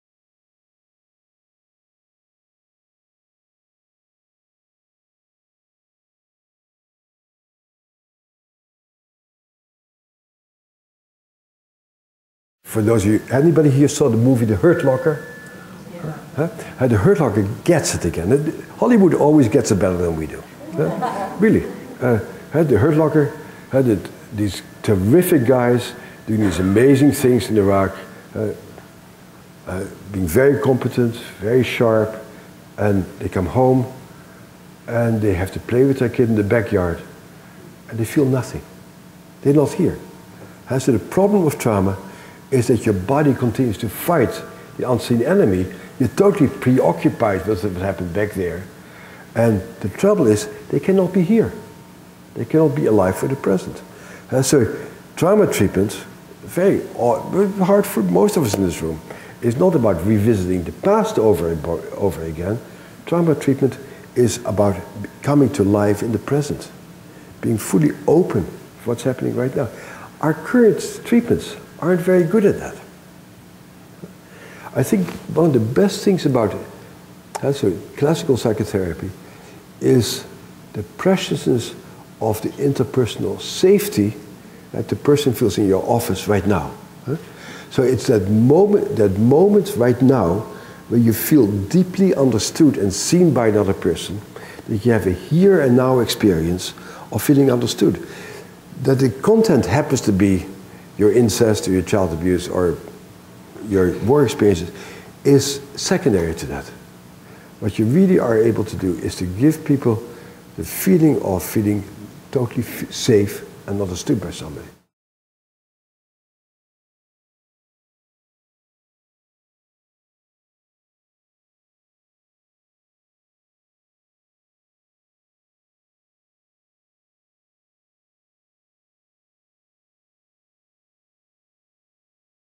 Video Excerpt of "Trauma, Attachment & Neuroscience" Seminar with Bessel van der Kolk, M.D.
This is a video excerpt featuring Bessel van der Kolk, M.D. from his video lecture entitled "Trauma, Attachment & Neuroscience with Bessel van der Kolk, M.D.". For details on the full video and to order, please visit CMI Education Institute's